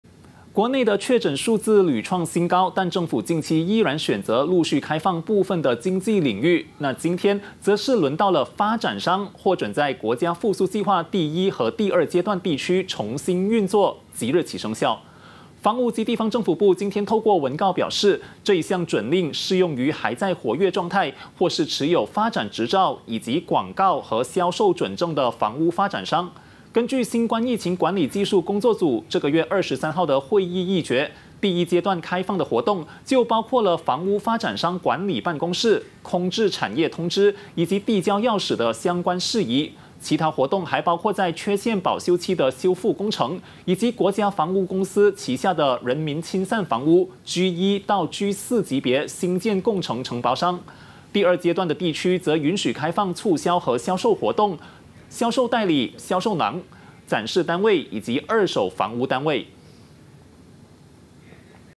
【复原计划首阶允房产领域复业 次阶准促销与展示】
国内确诊数字屡创新高，但政府近期依然选择陆续开放部分经济领域。今天则是轮到发展商获准在国家复苏计划第一和第二阶段地区重新运作，即日起生效！